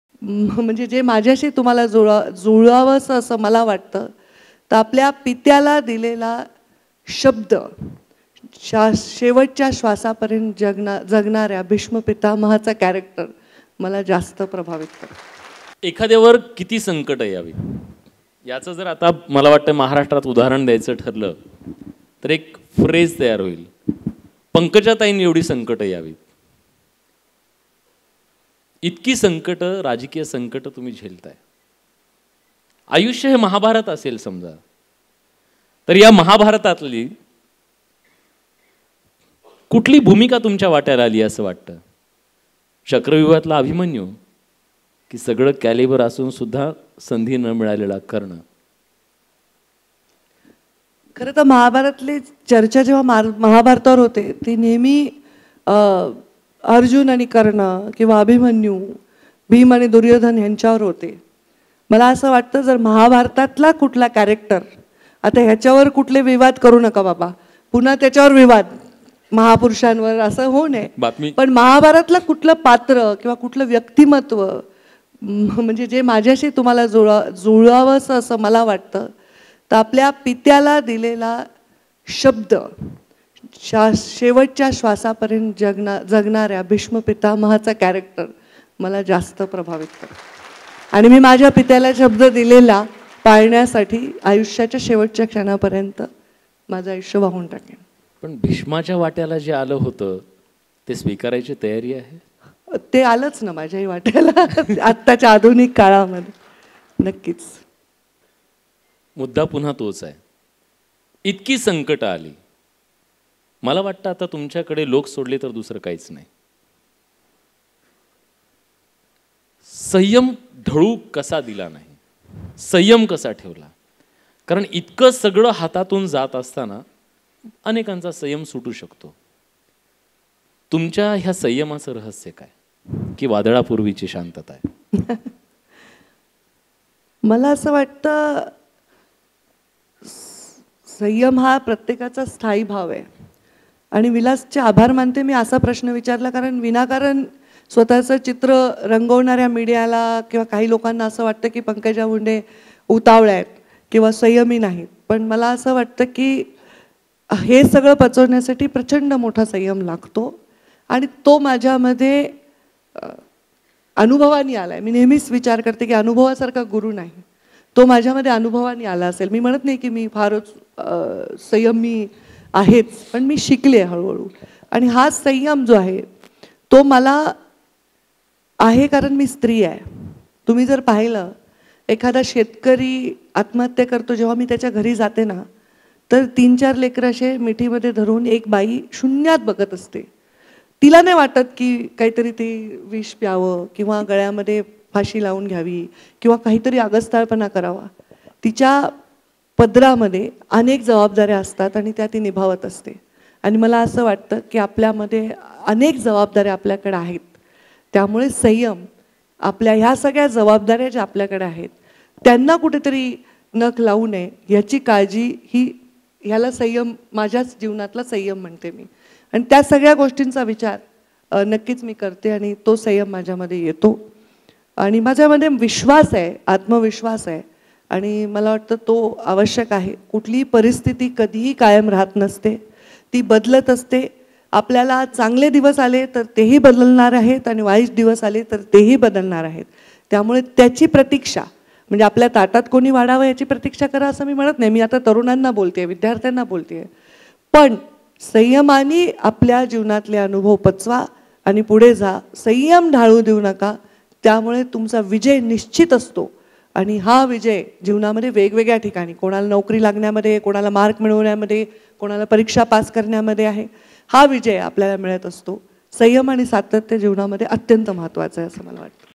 Pankaja Munde Nashik Interview | पंकजा म्हणतात,भीष्म पीतामह कॅरेक्टर मला शोभतं! | Maharashtra Times
नाशिकमध्ये विद्यार्थ्यांना करिअर विषयी मार्गदर्शनासाठी विद्यार्थी परिषद आयोजित केली होती.
या विद्यार्थी परिषद भाजपा नेत्या पंकजा मुंडे यांची मुलाखत पार पडली.
यावेळी भाजपा नेत्या पंकजा मुंडे यांनी विविध मुद्द्यांवर भाष्य केलं आहे.
भीष्माच्या जे वाट्याला आलं ते स्वीकारण्याची तयारी आहे का? असा प्रश्न यावेळी विचारला.
तसंच महाभारतातील भीष्म पीतामह कॅरेक्टर मला शोभतं, असं त्या म्हणाल्या.
त्यावर बोलताना त्या म्हणाल्या, भीष्माच्या वाट्याला जे आलं ते माझ्याही वाट्याला आलं.
संयमाने आपल्या आयुष्यावर विजय मिळवा, असंही यावेळी पंकजा मुंडे म्हणाल्या.